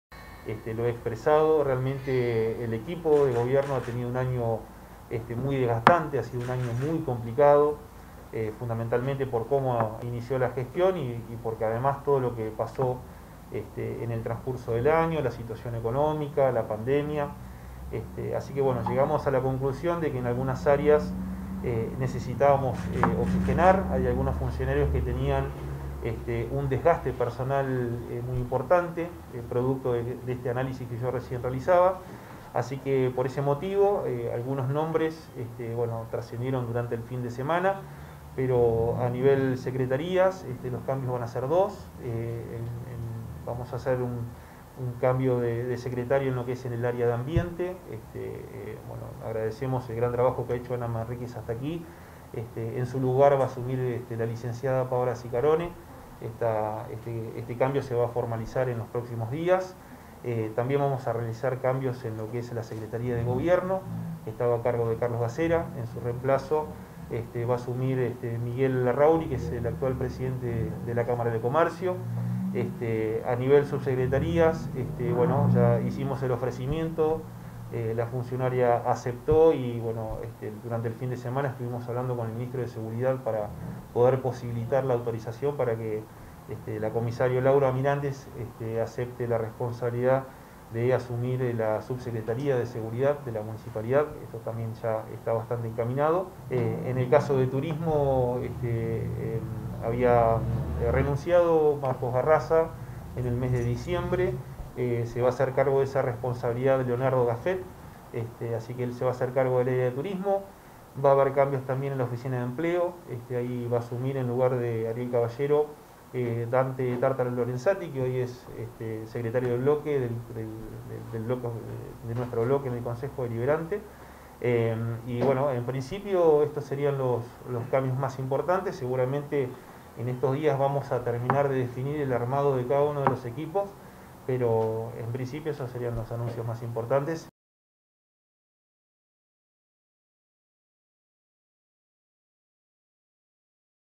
Cambios para "oxigenar" el Gabinete
El intendente de Rawson, Damián Biss anunció las modificaciones en su equipo de trabajo.